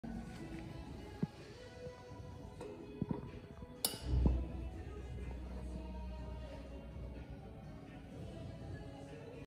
Start Your Day Right with the Barisieur Black Coffee Alarm Brewer
Rise and shine, coffee lovers! ☀️☕️ Say goodbye to boring mornings and hello to the Barisieur Black Coffee Alarm Brewer! 🌟 With its sleek design and innovative brewing technology, you can wake up to the perfect cup of coffee every day. 🙌 Whether you prefer a strong espresso or a smooth latte, the Barisieur has got you covered. Don't settle for ordinary coffee - start your day off right with the Barisieur Black Coffee Alarm Brewer! 💯